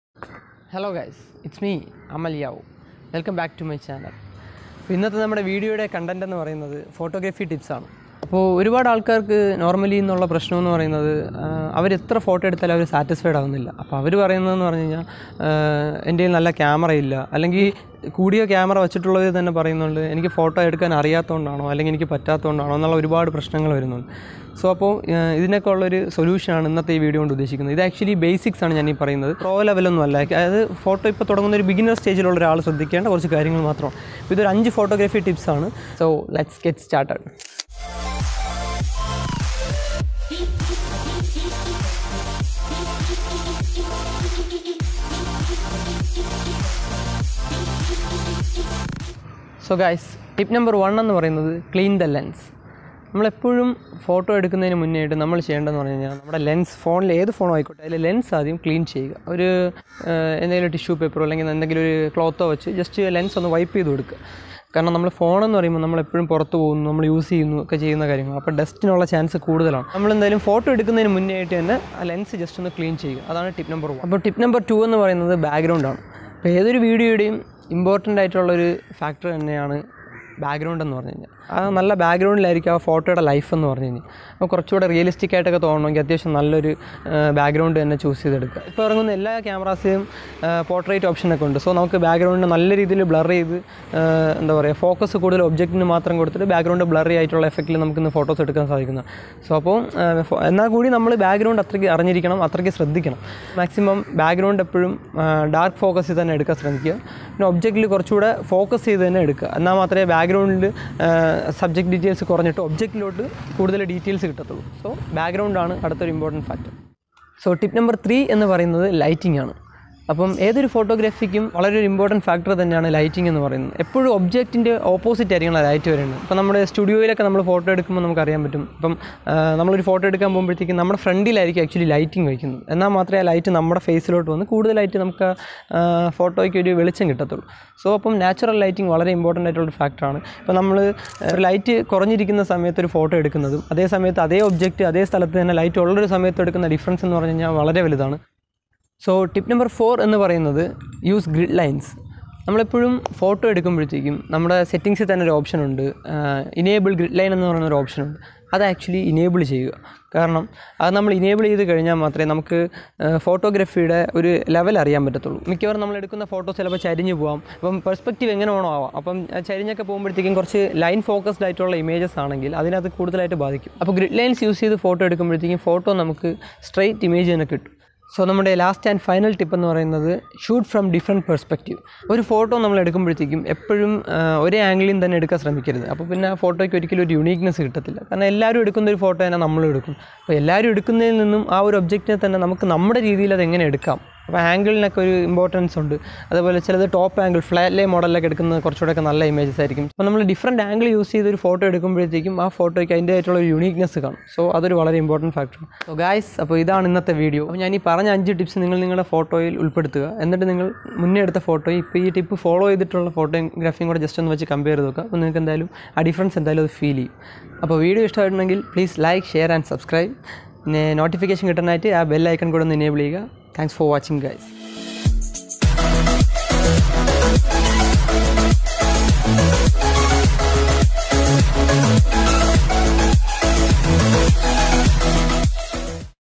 5 Mobile Photography Tips || For Beginners || Malayalam
Hi Everyone,

               This Video is for the Beginners in mobile photography. Video contains five effective tips for the beginners along with brief video clips as well.
So watch the complete video and keep support and Love. - yao